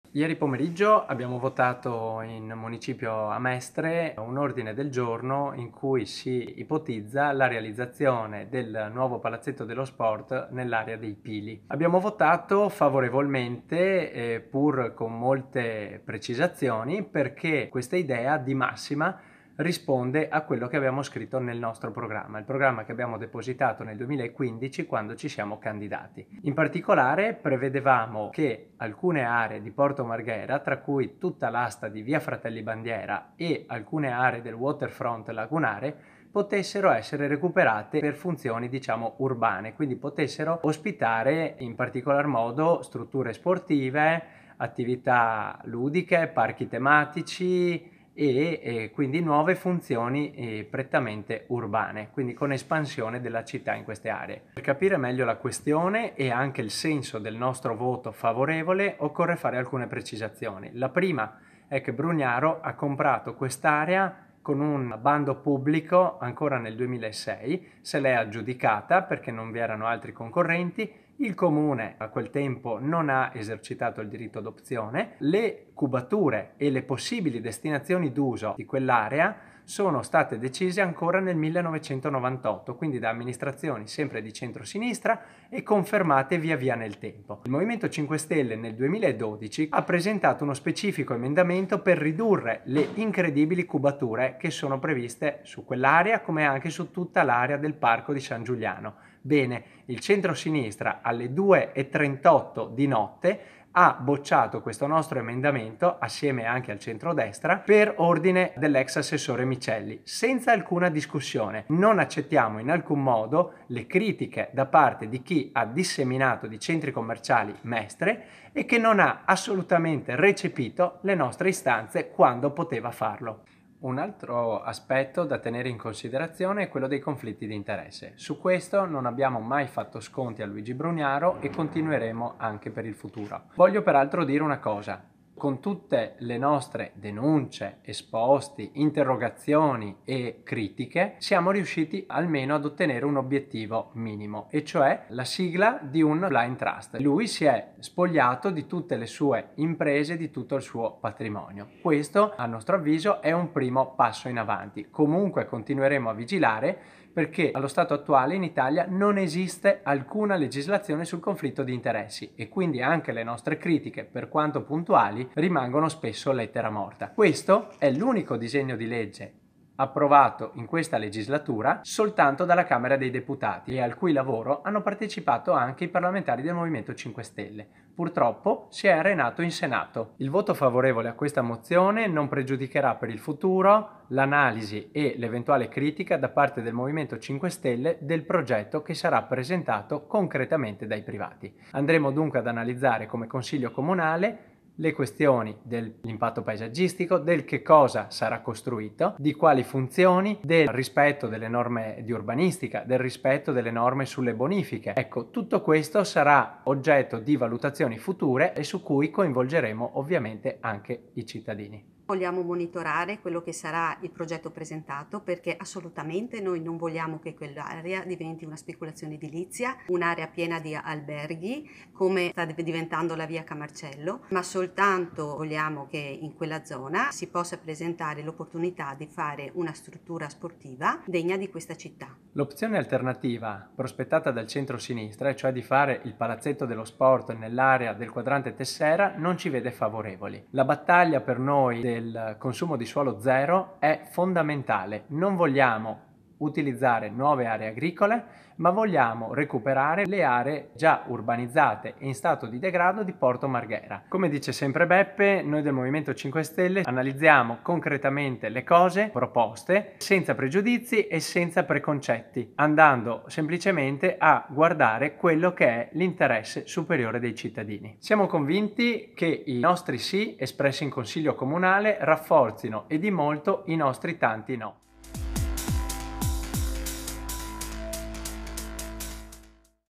Palazzetto ai Pili: le ragioni del M5S
Le spiegazioni dei nostri Consiglieri sul voto per il Palazzetto ai Pili